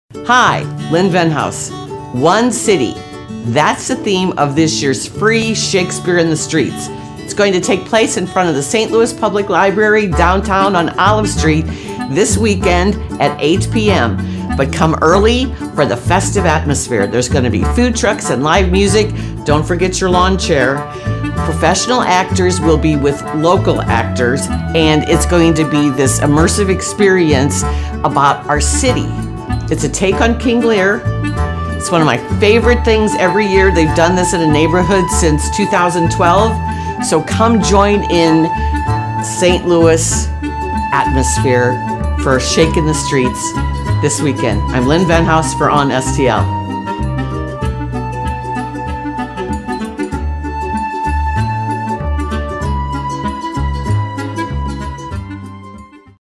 Lynn Venhaus - 9 -13-17
Lynn Venhaus tells us about something fun to check out this weekend, that we bet you didn't know was happening. This week it's Shakespeare in the Street at the St. Louis Public Library (Downtown).